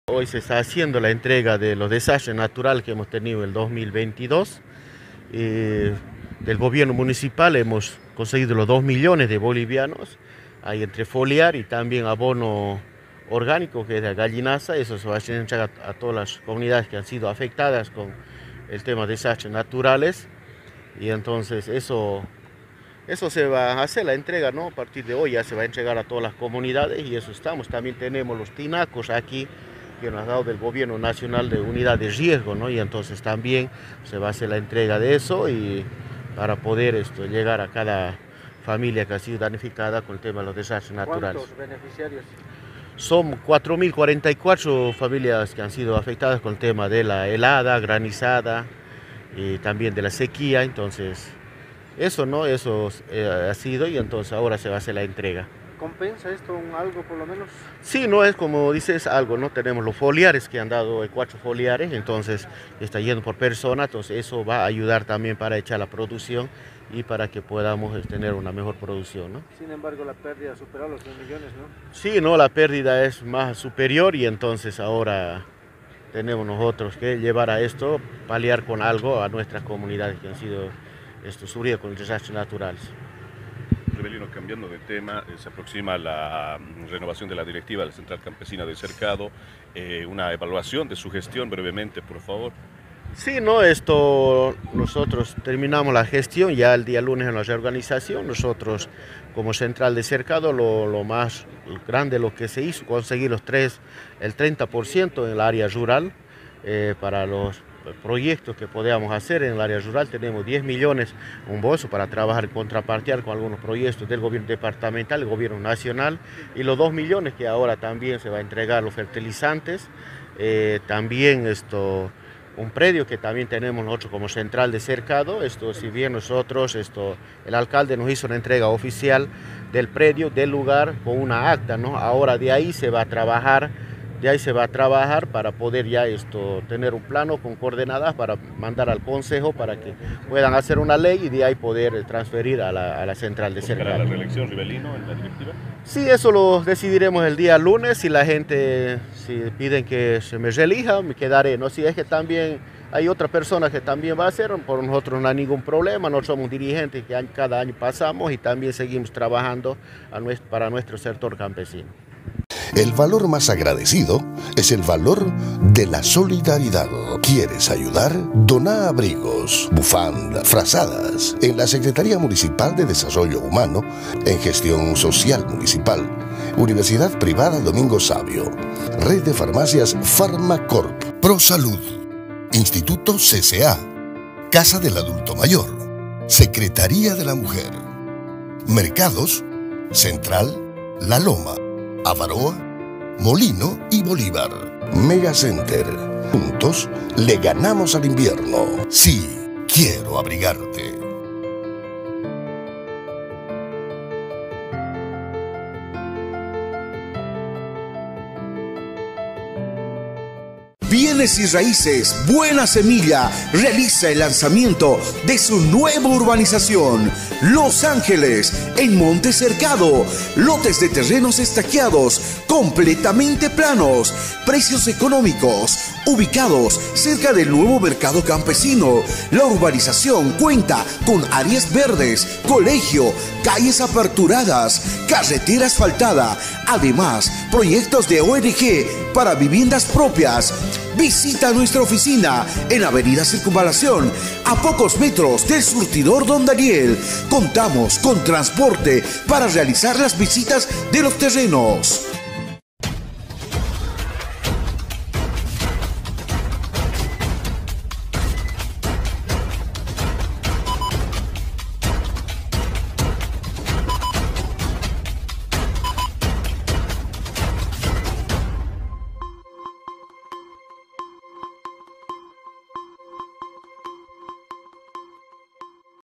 ALCALDIA DOTA DE INSUMOS PARA EL SECTOR CAMPESINO
ALCALDIA DOTA DE INSUMOS PARA EL SECTOR CAMPESINO E INFORMAN QUE LAS ELECCIONES DE LA CENTRAL CAMPESINA DE CERCADO SERÁ EL 29 DE MAYO